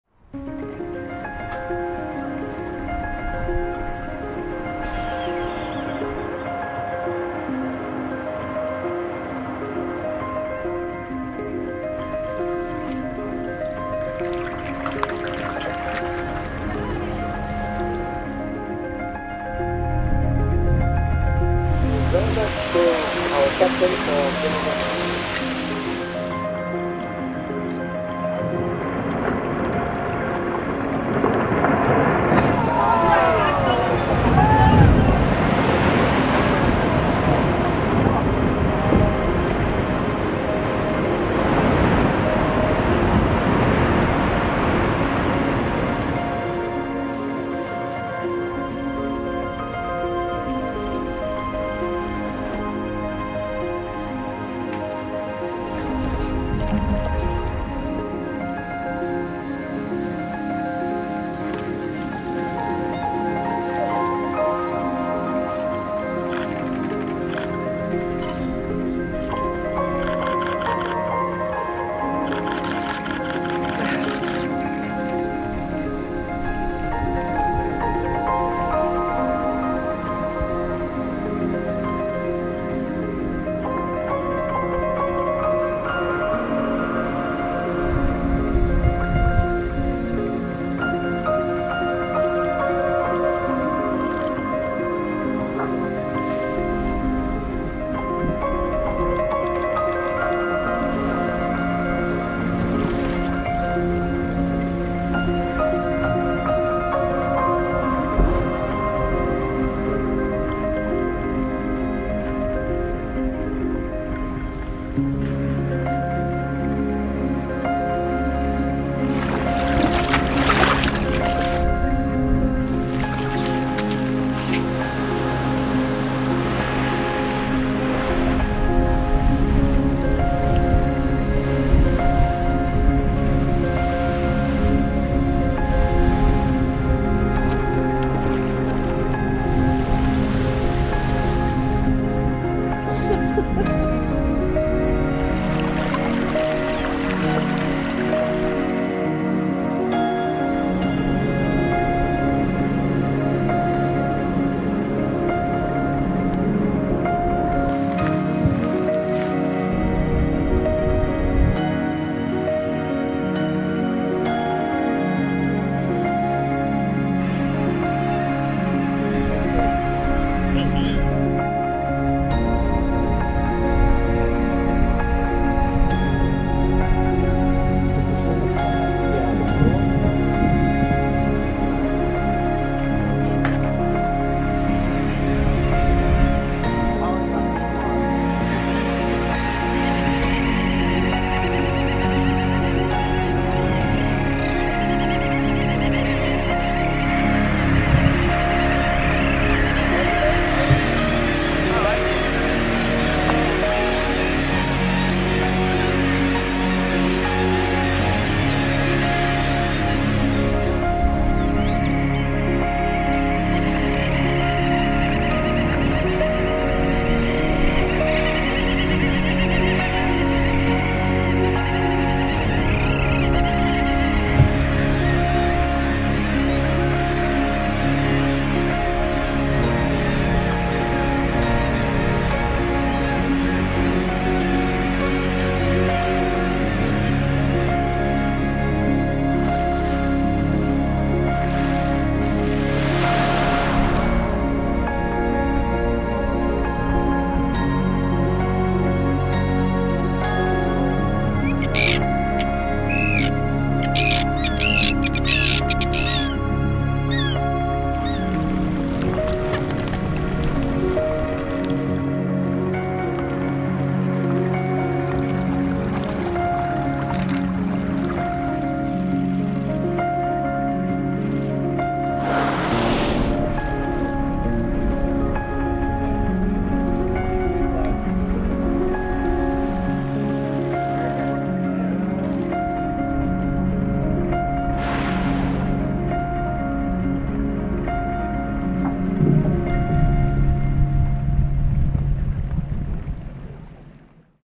Arctic Svalbard 2017
A month exploring Arctic Svalbard on the National Geographic Explorer in search of Polar Bear. In a land where the sun never set for my entire assignment, we chased the retreating sea ice to well above 80 degrees North latitude. Arctic fox, Svalbard Reindeer and millions of sea birds, all under a dense mat of micro vegetation making the most of the short but intense Arctic Summer.